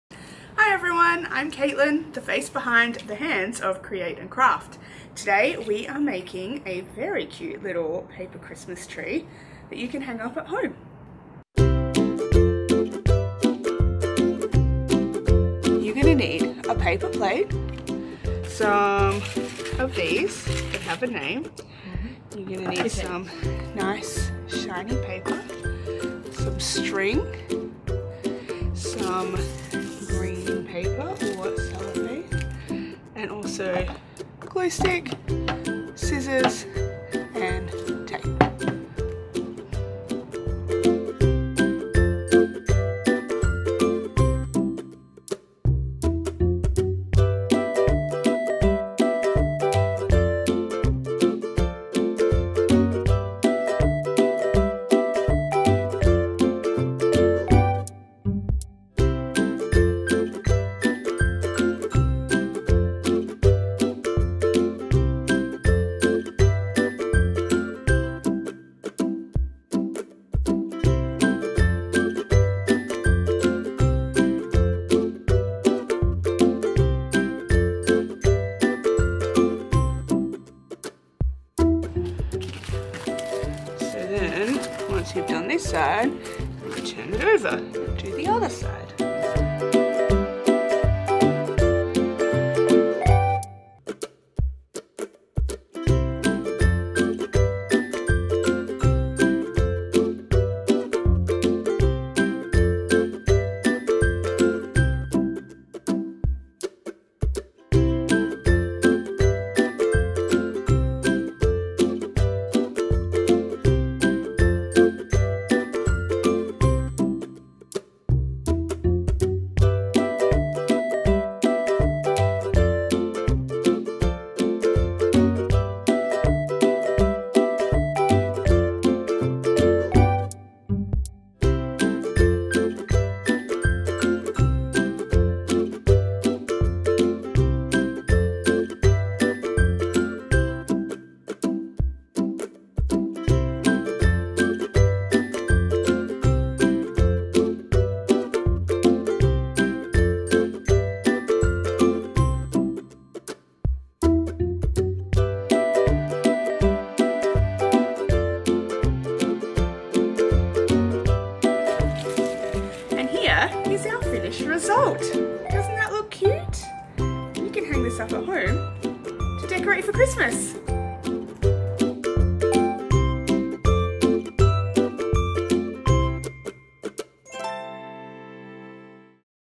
Create and Craft - Paper plate Christmas tree
Caitlin creates a quick and easy paper plate Christmas tree designed for ages 2-5 years to make with a carer.

No template is needed for this one.  All you will need is a thin paper plate, some green tissue/crepe paper/cellophane, a piece of string or ribbon, some coloured or sparkly card and some bits and pieces to decorate the tree. We used patty pans, but you can use anything that is safe for a child to use.
A pair of scissors, a glue stick and some sticky tape will help you stick it all together.

We have books to accompany this craft listed on our and you can access Storybox and have a book read to you via our eLibrary